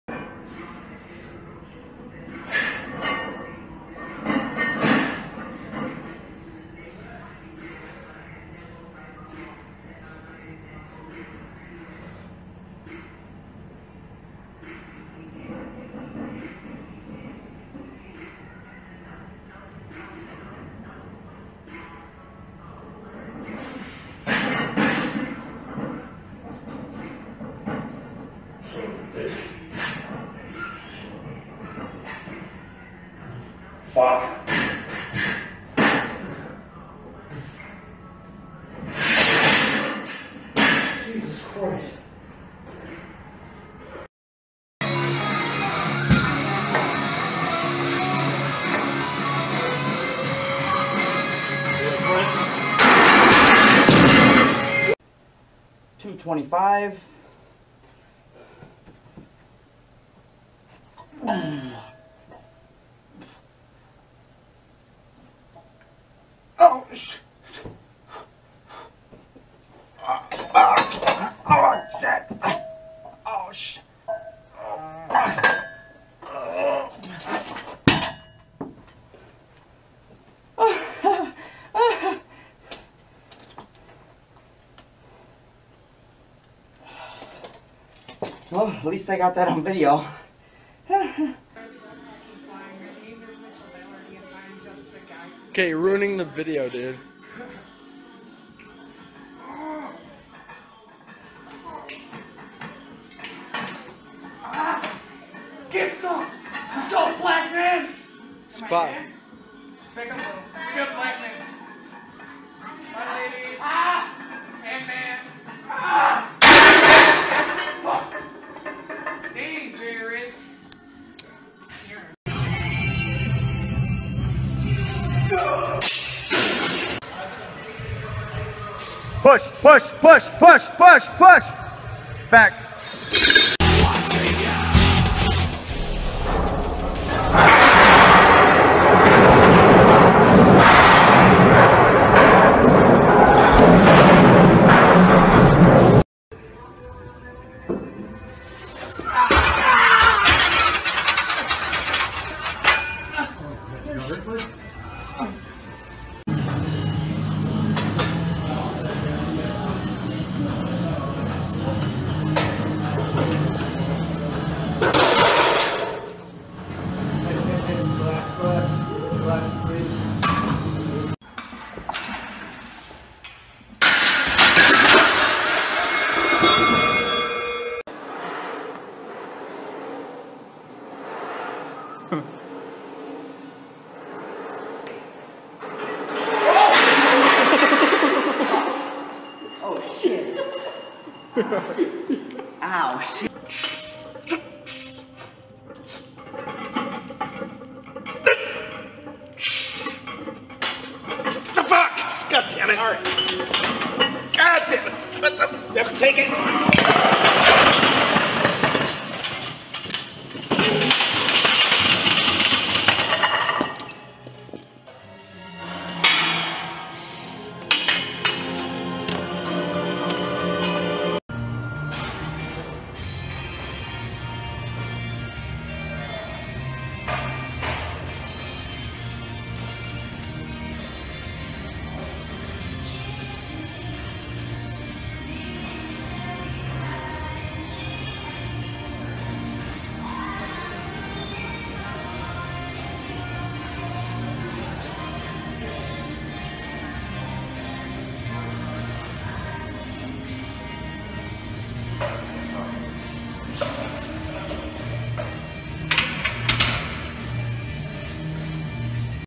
TOP SQUAT FAIL COMPILATION
Hello, this video is about squat fails. Enjoy!